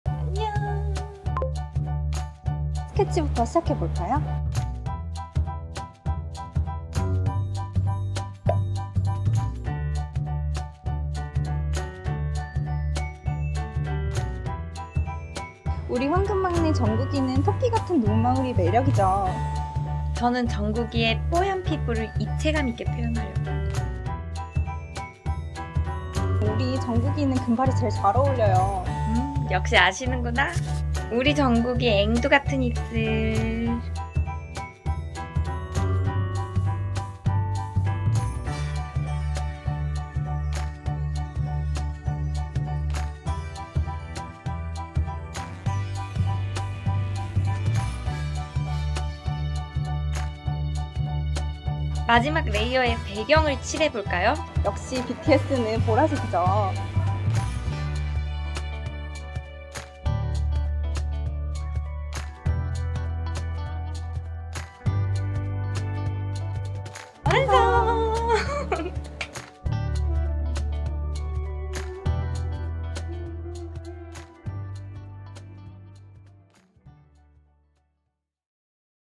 아트봉봉 선생님들의 정국 반반 그리기! ㅣ방탄소년단 정국ㅣDrawing BTS Jung Kook
BTS 찐 팬 아트봉봉 선생님들은 어떻게 놀까요?

아트봉봉의 금손!
워니쌤과 로원쌤이 주말에 심심해졌어요🫢
그래서 시작 된 아트봉봉으로 정국 함께 그리기!

아트봉봉은 다른 장소에서도 한 화면에 그림을 함께 그릴 수 있답니다😄

그러면 선생님 스타일 대로 반반 그린 정국 보러가실래요?

😊 문의
카카오톡 채널 검색 bonbonedu